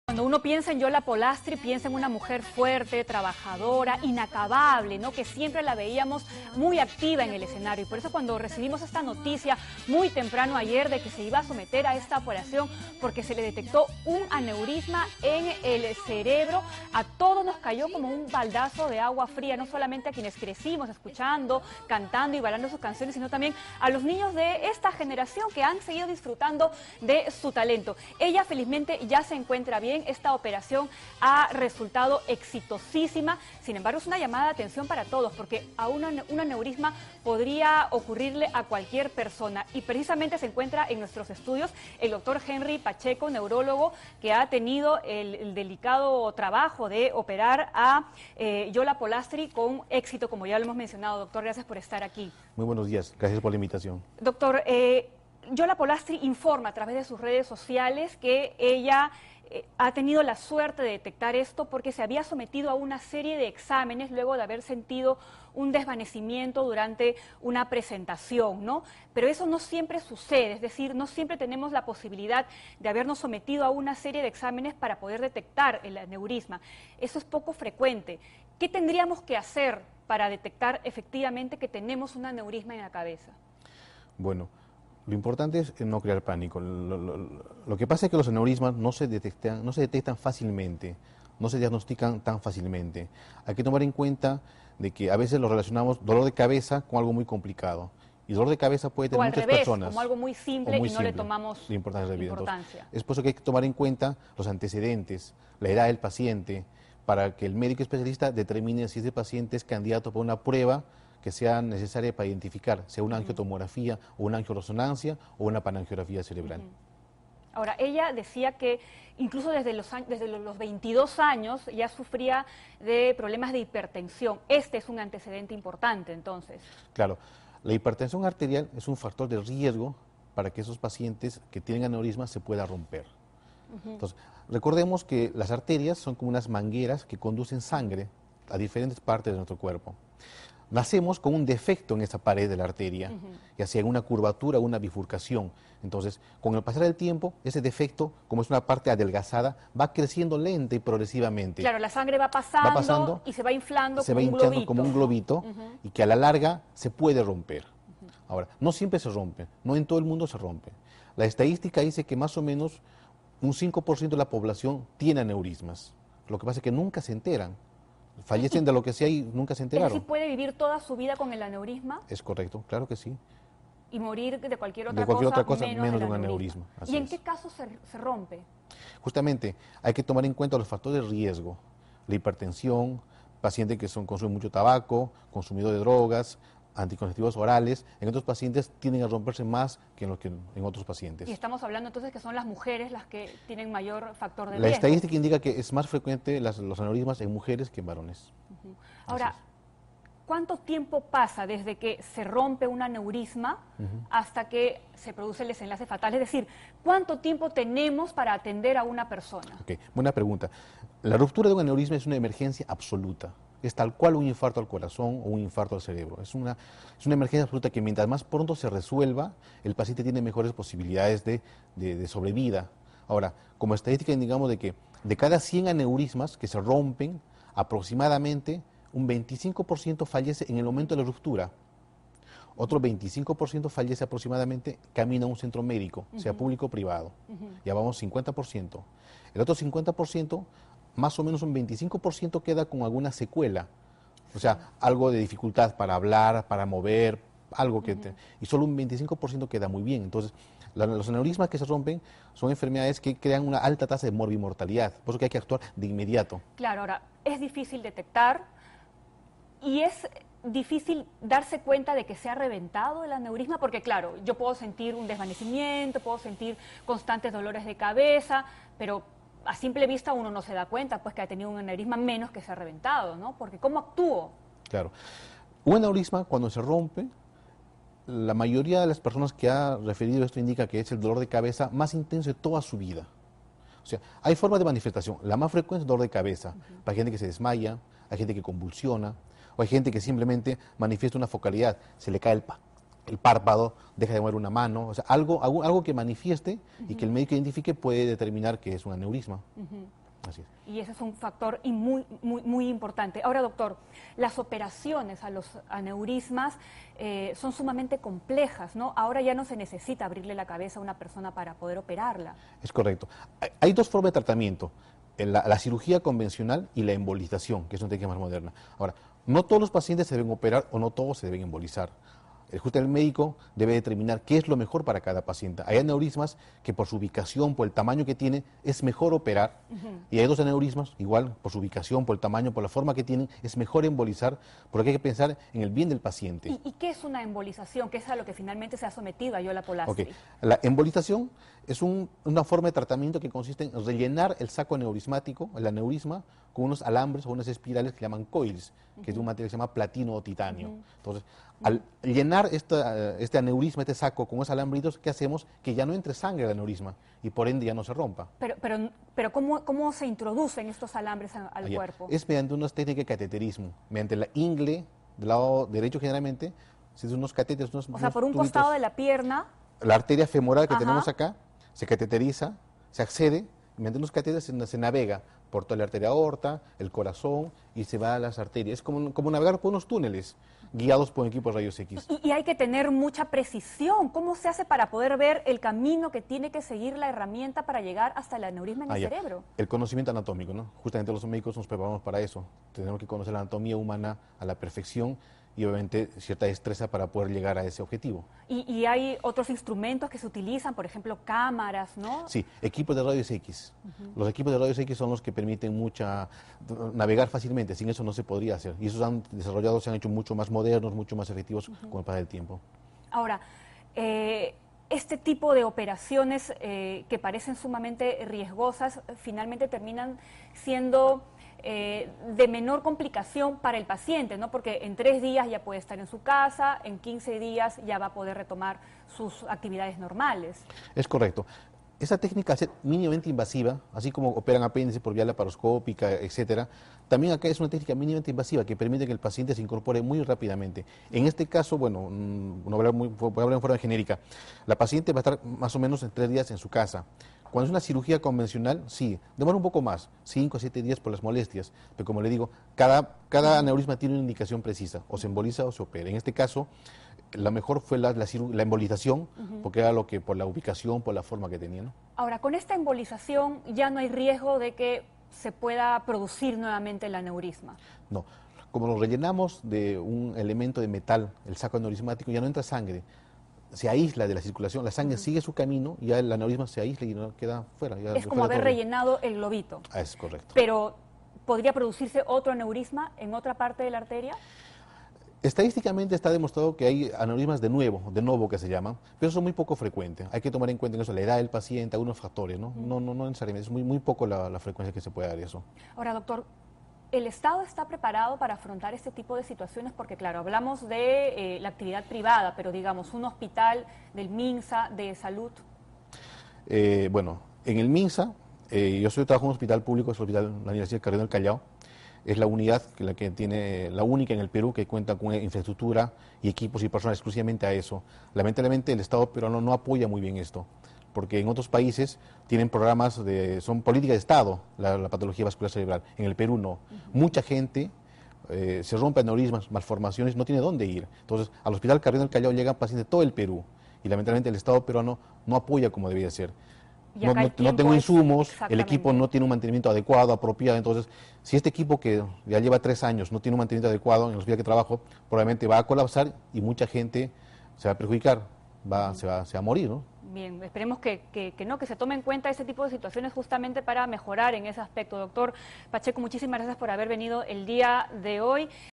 Yola Polastry: conozca los posibles síntomas del aneurisma cerebral
“Tengo una bomba de tiempo en mi cabeza”, fue como describió ayer la destacada animadora infantil Yola Polastry antes de ser operada a causa de un aneurisma cerebra​l. La intervención a la recordada &#39;chica de la tele&#39; fue exitosa porque fue detectada a tiempo; sin embargo, esta patología asintomática a veces puede ser indetectable.

A los estudios de Buenos Días Perú llegó el doctor Henry Pachecho, neurólogo que operó con éxito a la animadora infantil, quien señaló que los aneurismas no se detectan a tiempo y explicó algunos de los síntomas de esta patología asintomática. El especialista indicó que los aneurismas no se detectan fácilmente porque es confundido con dolores de cabeza.

Por ello, indicó que el médico especialista debe tomar en cuenta los antecedentes del paciente, la edad para que se puede identificar mediante una angiotomografía, angioresonancia o también una panangiografía cerebral. Además indicó que es un riesgo esta patología en pacientes con hipertensión arterial.